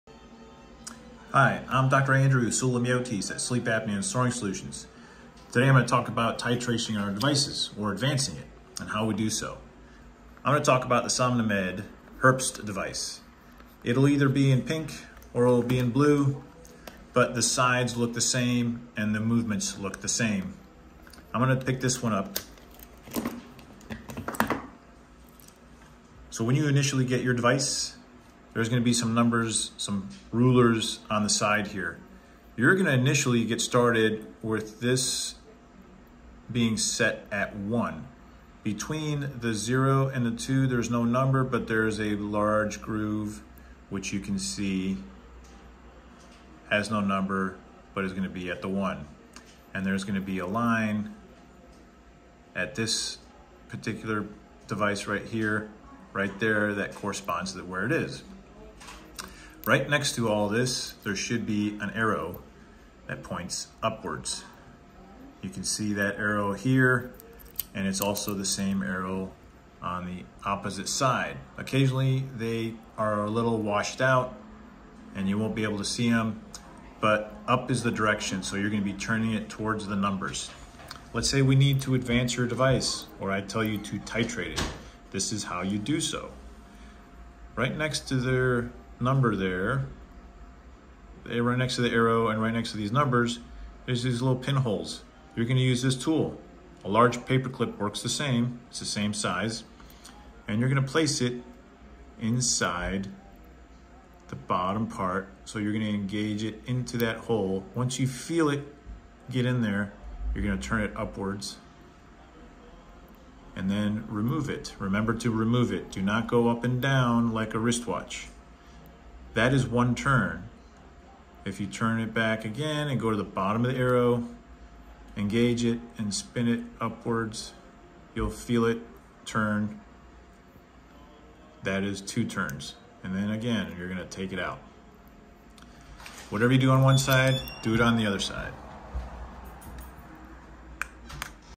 Somnomed Herbst device titration, advancement. Oral Appliance Therapy for sleep apnea.
This video describes how to titrate or advance a Somnomed Herbst device. This is one style Oral Appliance Therapy for the treatment of sleep apnea and snoring.